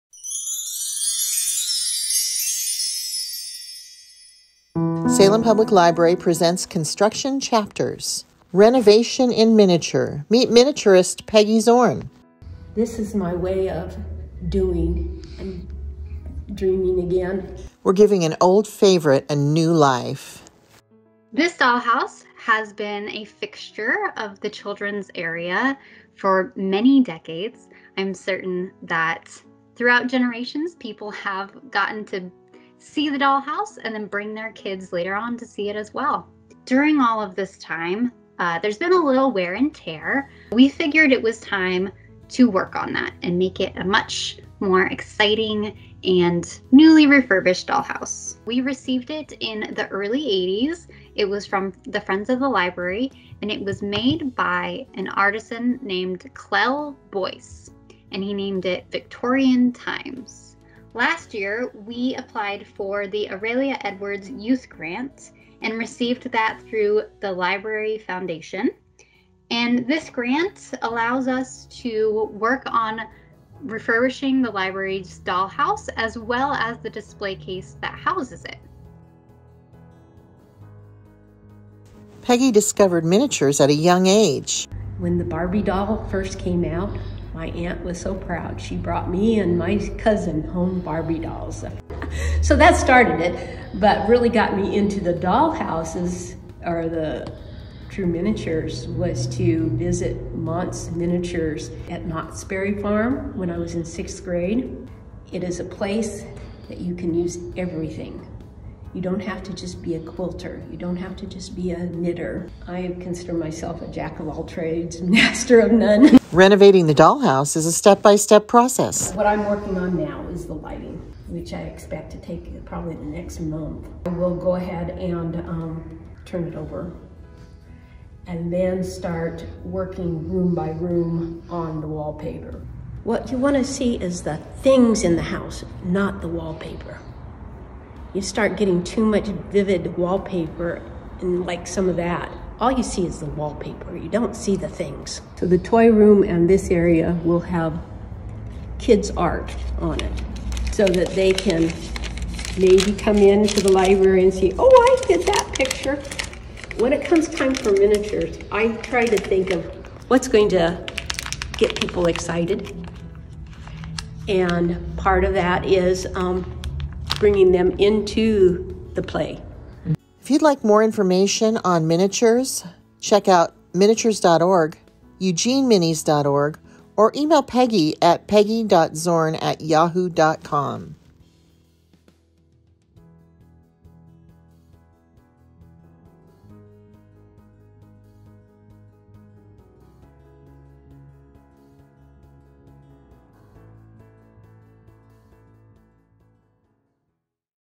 Construction Chapters Renovation in Miniature
While the Salem Public Library is being renovated, so is its beloved dollhouse thanks to an Aurelia Edwards Grant from the Salem Public Library Foundation. Longtime miniaturist Peggy Zorn was selected for the renovation project.

Zorn shares her love of miniature dollhouses and the renovation process, while Hannah Bostrom, Librarian at Salem Public Library